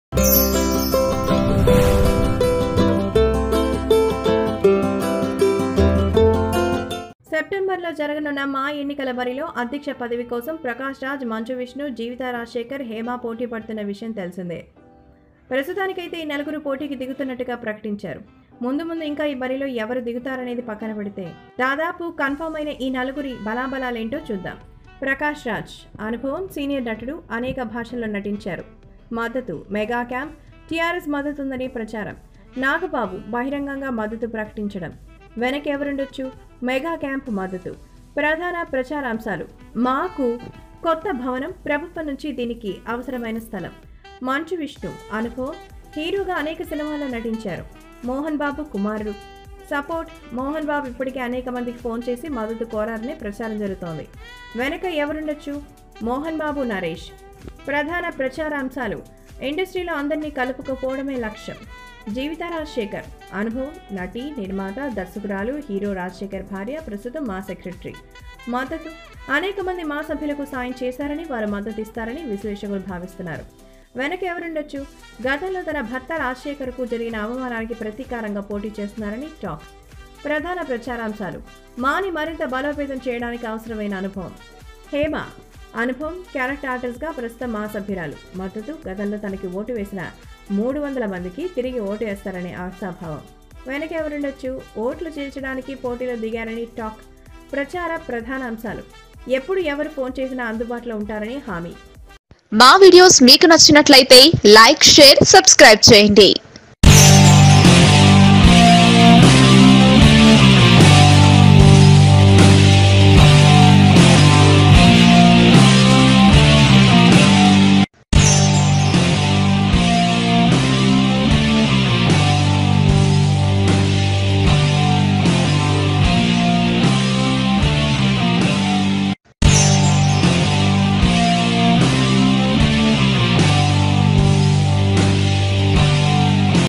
MAA Elections 2021 Contestants Strength | Prakash Raj | Manchu Vishnu | Jeevitha | Hema |News Mantra
When Prakash Raj announced his decision openly, Naga Babu wished him, and this clearly showed that the mega family was in support of Prakash Raj. Manchu Vishnu they have sought the support of senior star Krishna and Krishnam Raju instead. Meanwhile, Jeevitha Rajasekhar too announced she would be contesting for the president post. Surprisingly, Hema, who held many posts in MAA, has also announced that she will be contesting for the post.